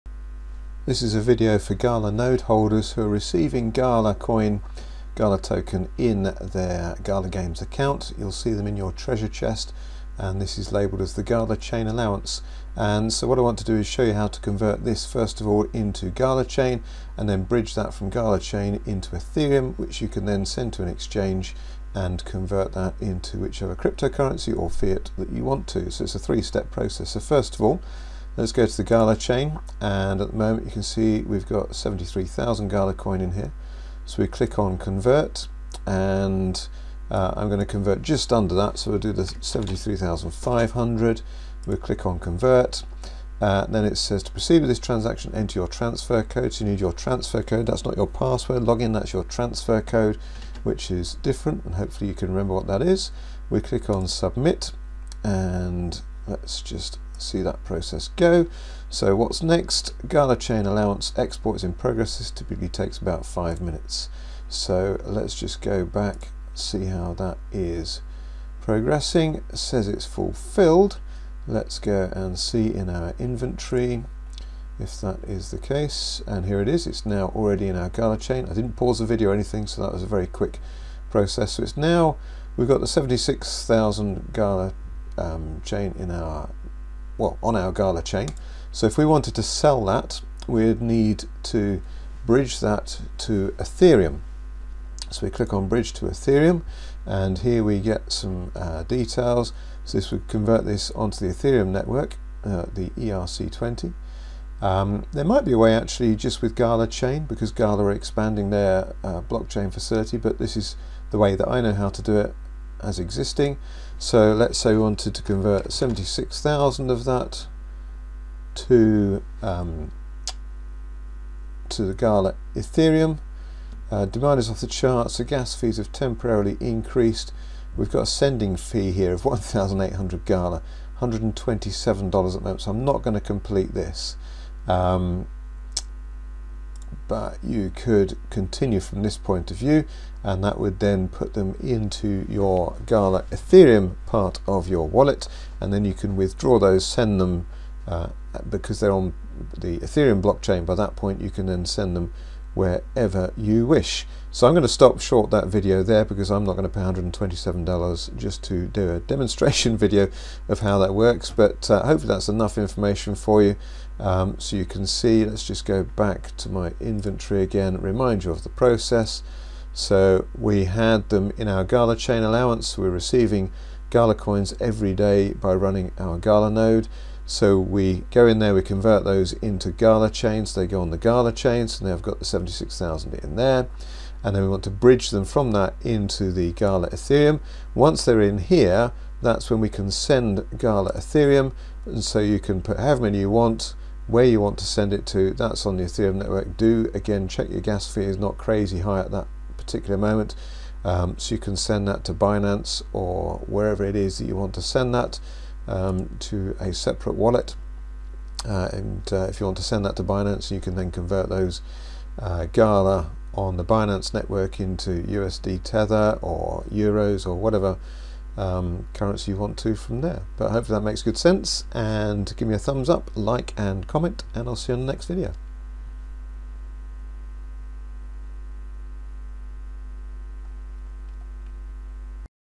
How to convert your gala coins into real money (tutorial video for node holders)
A short tutorial video for Gala Node holders, showing how to convert your Gala Chain Allowance tokens/coins into real money.

It's a 3 step process.
Step 1: CONVERT your Gala Chain Allowance Coins (in your treasure chest) into  Gala Chain coins.
Step 2: BRIDGE your Gala Chain Coins to the Ethereum network (gala coins ETH)
Step 3: SEND your Gala ETH coins to where you wish e.g. Binance exchange where you can convert them to whatever other currency you wish.